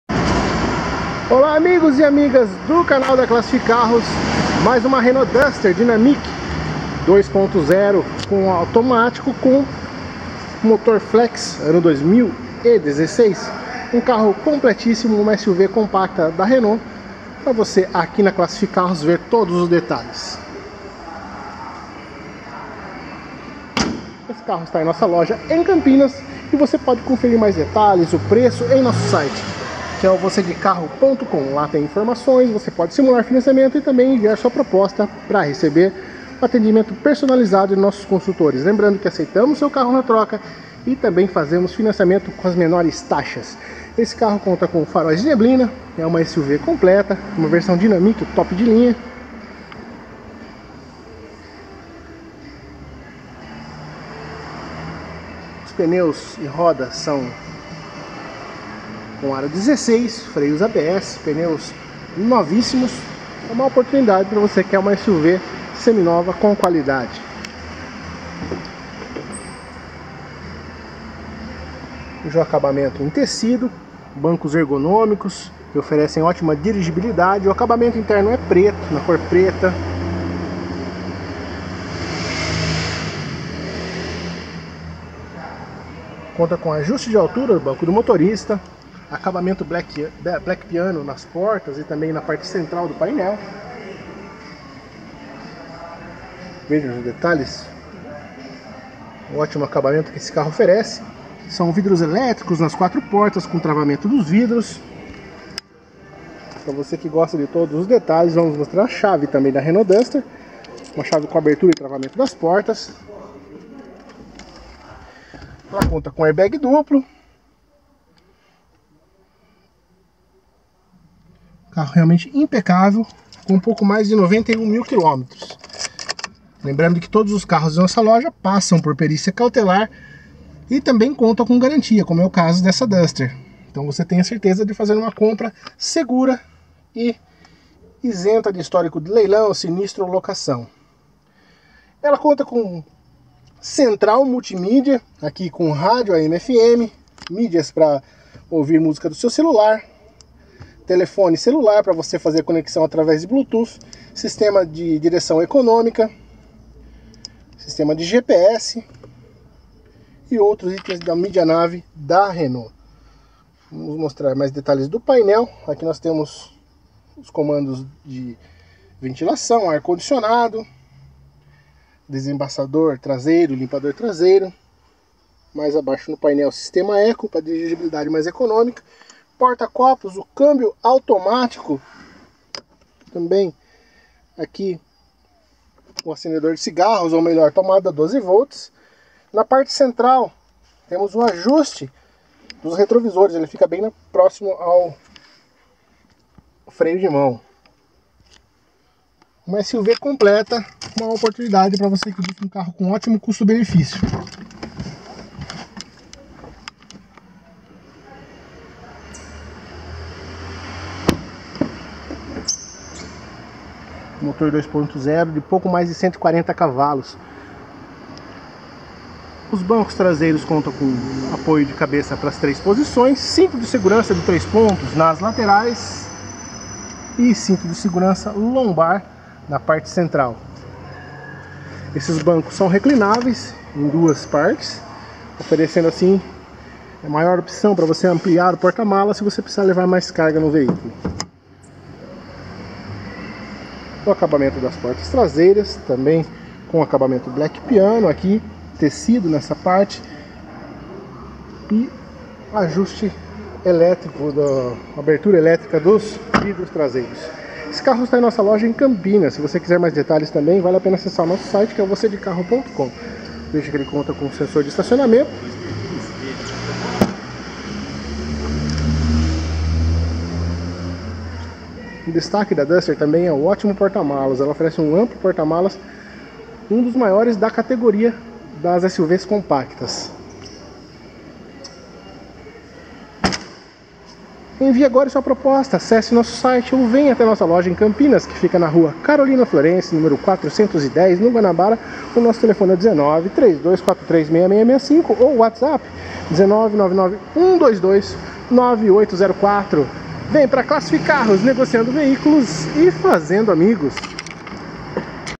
Renault Duster Dynamique 2.0 16v Automático (Flex) 2016
IMPECÁVEL, COM MANUAL E CHAVE RESERVA. SUV COMPLETA DE SÉRIE, DESIGN ROBUSTO E APRECIADA PELA RELAÇÃO CUSTO x BENEFÍCIO. VERSÃO COM MOTOR 2.0 FLEX (148cv) E CÂMBIO AUTOMÁTICO. CONTA COM RODAS 16, MULTIMÍDIA, COMANDOS NO VOLANTE, GPS E BLUETOOTH